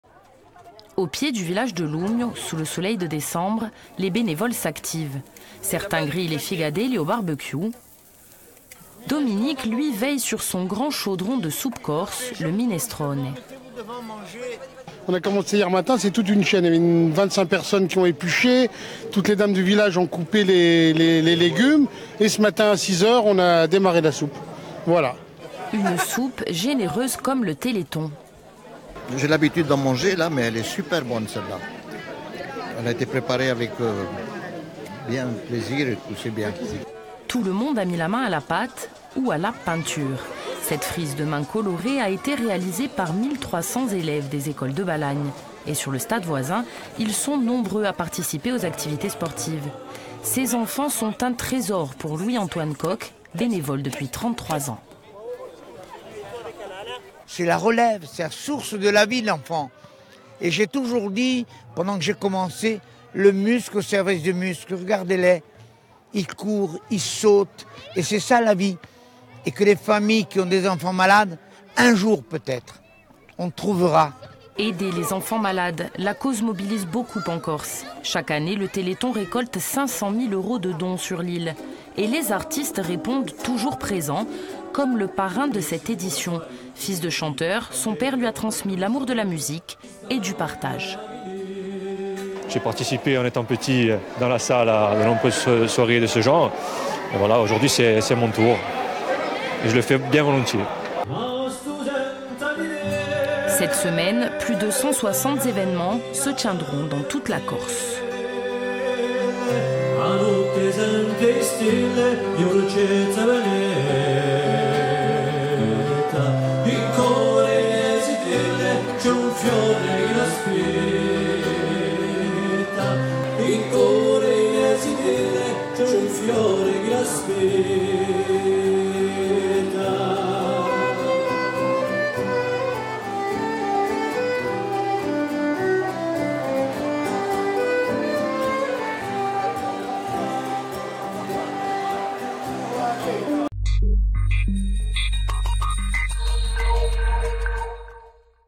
Lumio : une opération d’animations et de collecte en faveur du Téléthon
La Corse se distingue par sa générosité : avec environ 500 000 euros de dons par an en faveur du Téléthon. L’édition 2018 se tiendra le week-end prochain, mais dès ce samedi, une vaste opération d'animations et de collecte a été menée à Lumio.

Intervenants - Dominique Casta, Bénévole ; Louis-Antoine Coque
Coordinateur régional AFM ; Petru Santu Guelfucci, Parrain du téléthon 2018.
Equipe - Maia Graziani ; Guillaume Leonetti ; Philippe Feretti.